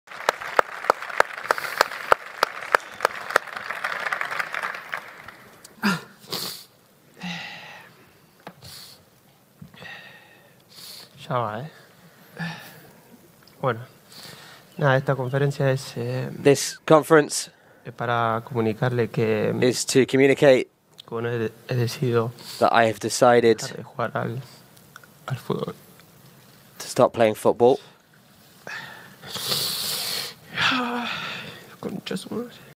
El KUN AGÜERO, emocionado en su despedida del fútbol
"Estoy aquí para comunicarles que he decidido dejar de jugar a fútbol…. " Lloraba y lloraba sin consuelo alguno. No podía articular palabra el Kun Agüero. "He decidido dejar de jugar, es un momento muy duro, pero sigo feliz igual por la decisión que tomé. Primero es mi salud. Ya saben porque tomé esta decisión. Es por el problema que tuve hace un mes y pico. Así que nada, estuve en buenas manos de los médicos. Me han dicho que lo mejor era que dejara de jugar. Tomé la decisión hace una semana. Hice todo lo posible para ver si había alguna esperanza. Pero no había muchas”, decía llorando sin parar, mientras era ovacionado en un silencioso Camp Nou, acompañado en el estrado por Joan Laporta.

"Estoy muy orgulloso por mi carrera, siempre soñé con jugar a fútbol desde los cinco años que toqué una pelota. Nunca pensé en llegar a Europa. Quiero agradecer a todos. A Independiente, donde me formé, luego al Atlético, apostaron por mi solo con 18 años, la gente del City, saben lo que siento por ese club, dejé lo mejor ahí… Me trataron muy bien. Y a la gente del Barça. Increíble, sabía que venía a uno de los mejores clubs del mundo. Las cosas pasan por algo. Argentina es lo que más amo. Agradecer a mi familia, a las personas que trabajaron conmigo", decía mientras el delantero, de 33 años, lloraba y lloraba.